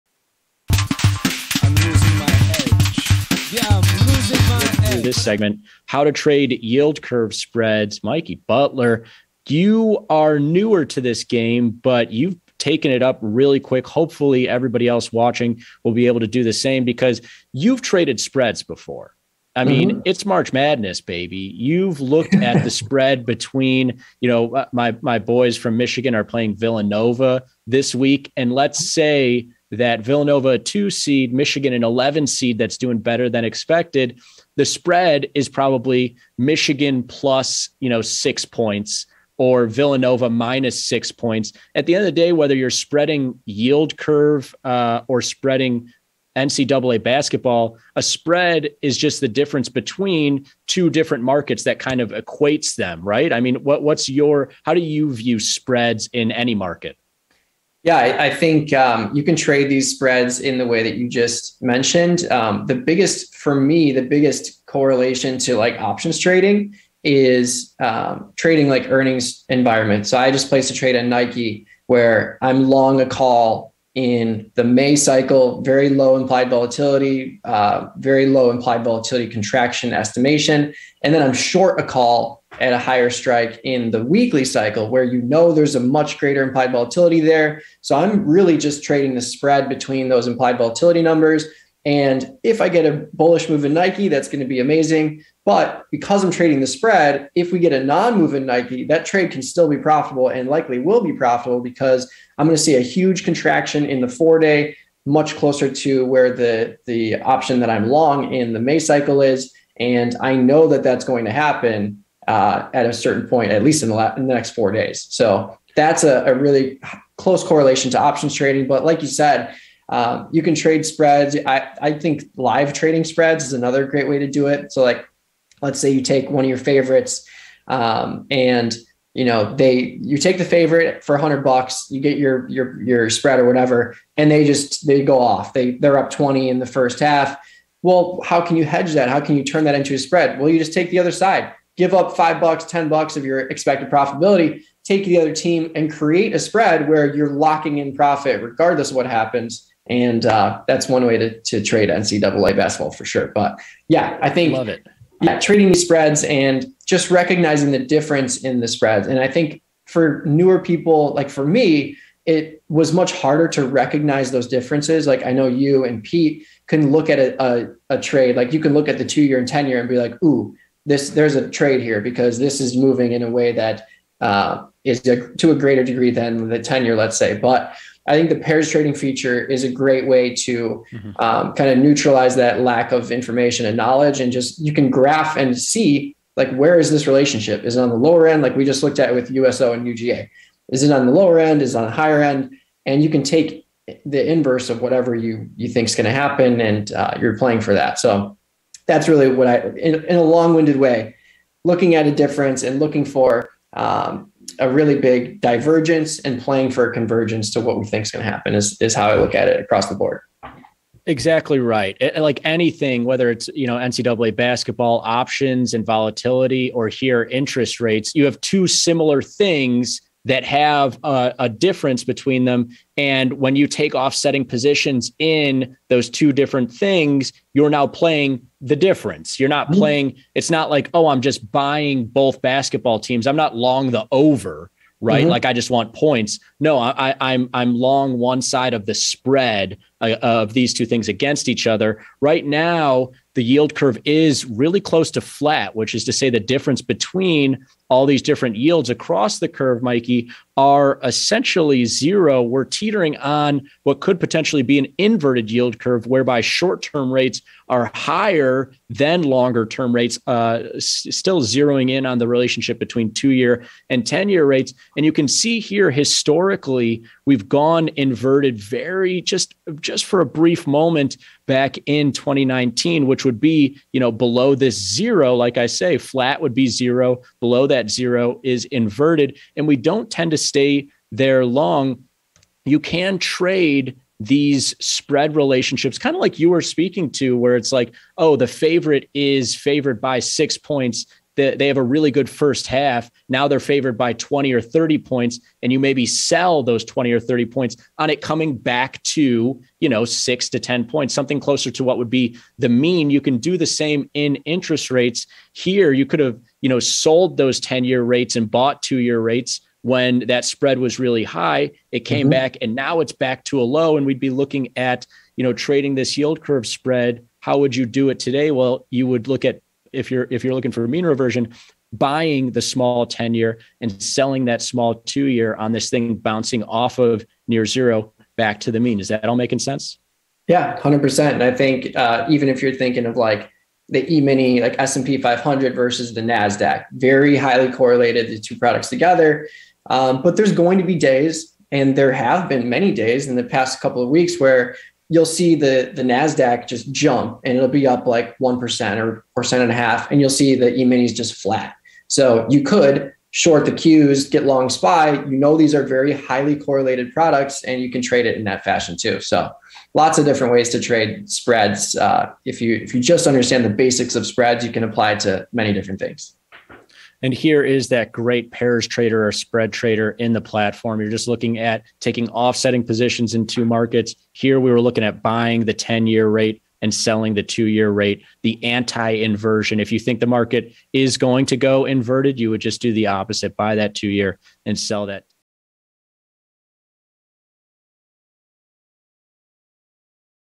How to Trade Yield Curve Spreads
Mikey and Frank show you how to trade yield curve spreads as 2 Year and 10 Year US Treasury interest rates near an inversion.

Yield curve inversions occur when short-term interest rates are higher than long-term rates, and they are often thought of as a sign of upcoming economic turbulence.

Derivatives dudes Mikey and Frank explore the potential for a yield curve inversion using Small Exchange futures and the Pairs Trader at tastyworks. The guys pit 10 Year US Treasury rates against 2 Year US Treasury rates to spread trade their difference and create a unique strategy that's at a price extreme.